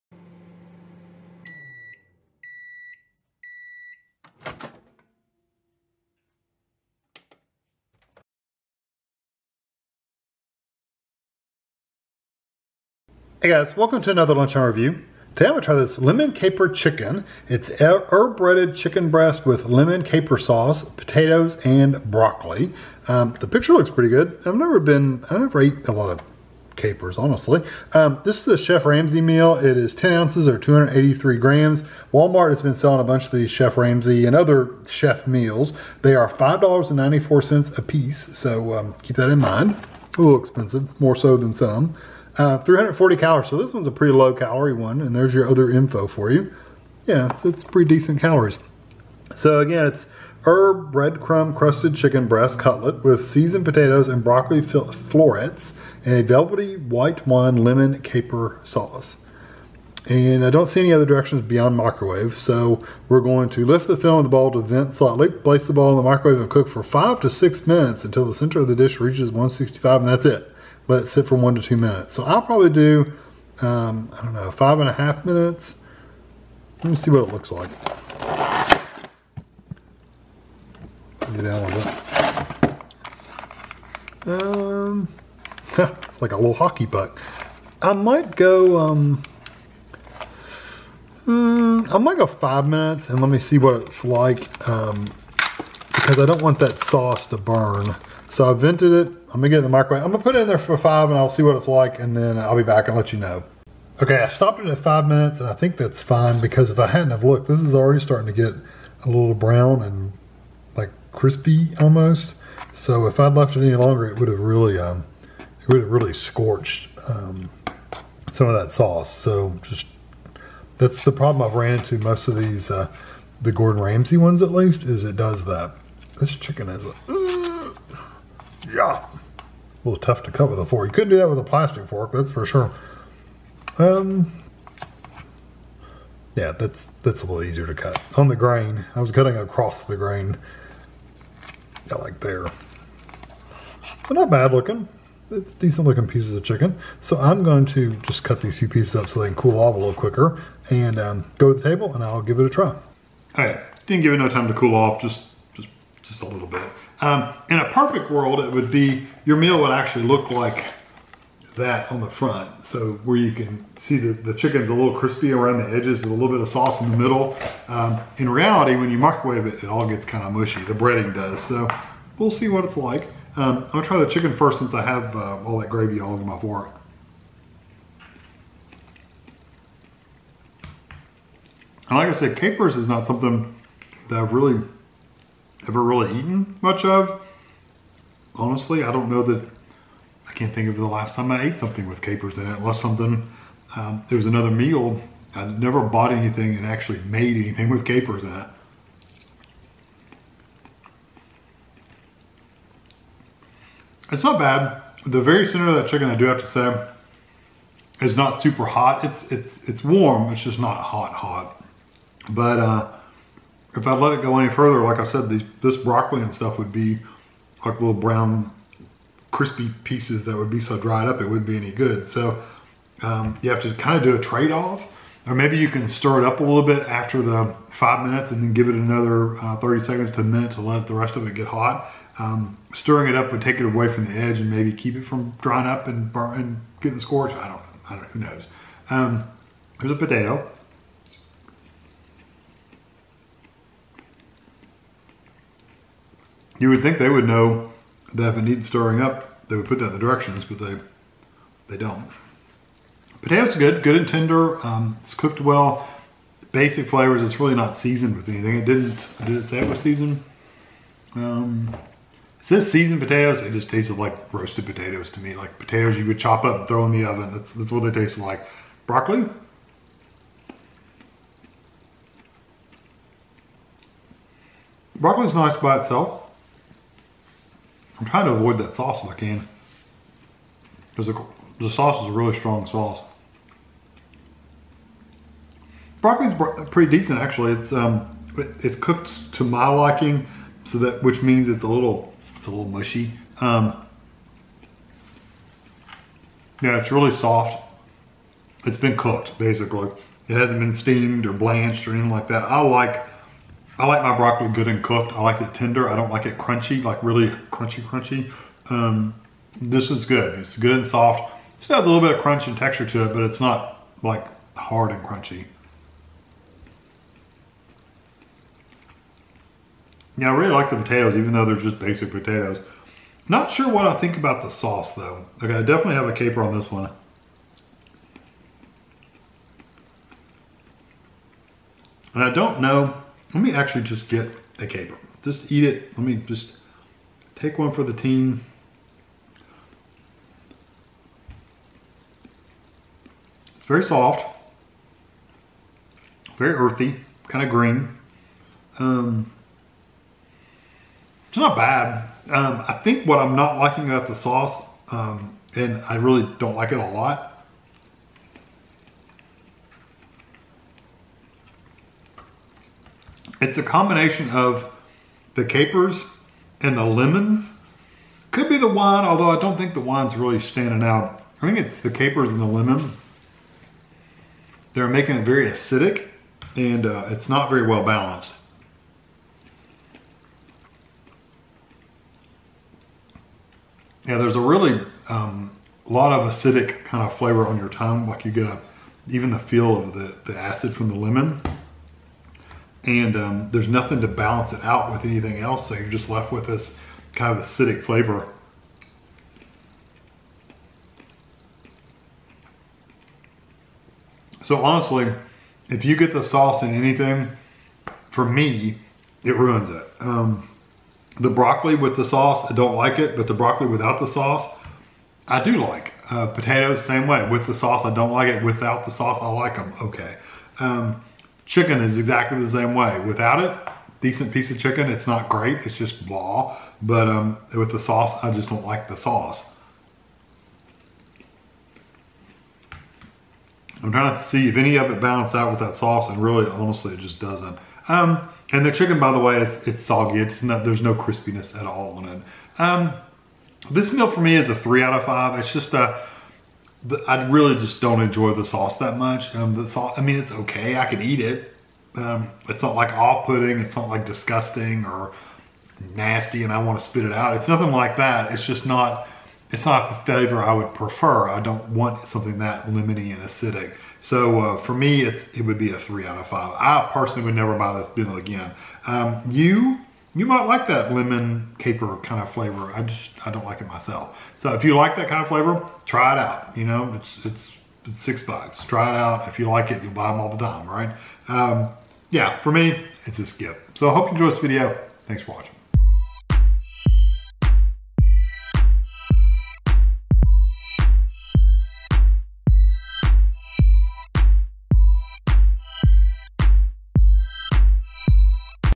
Chef Ramsay Lemon Caper Chicken Review
This is a taste test/review of the Lemon Caper Chicken By Chef Ramsay. It is herb breaded chicken breast with lemon caper sauce, potatoes and broccoli. It was $5.94 at Walmart. 1 bowl (283g) = 340 calories